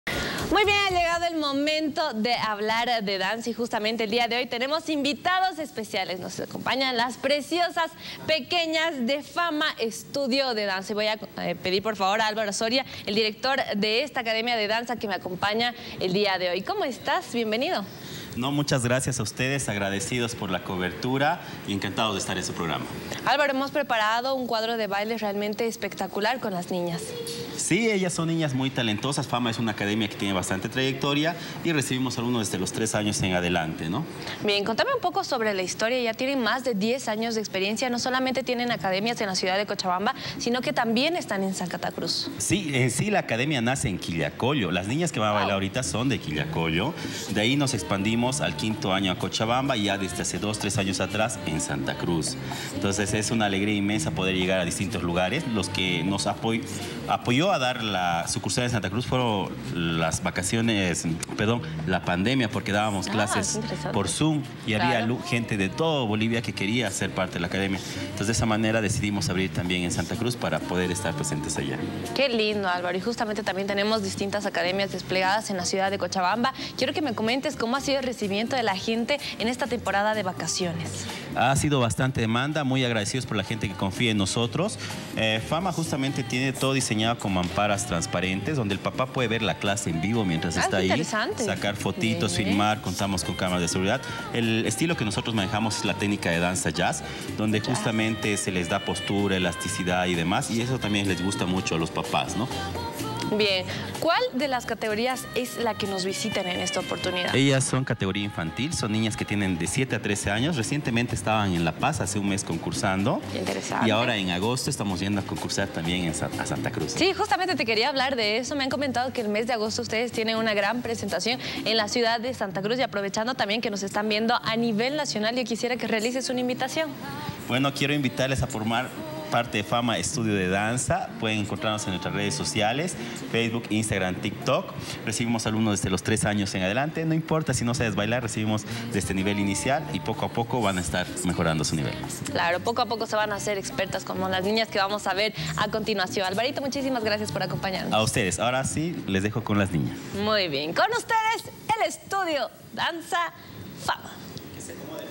La academia Fama visita el set de La 1ra
La academia de danza "Fama", presentó un espectacular cuadro de danza. Álvaro Soria, director de la academia, recalcó que "Fama" tiene presencia a nivel nacional, también señaló el éxito de los cursos vacacionales.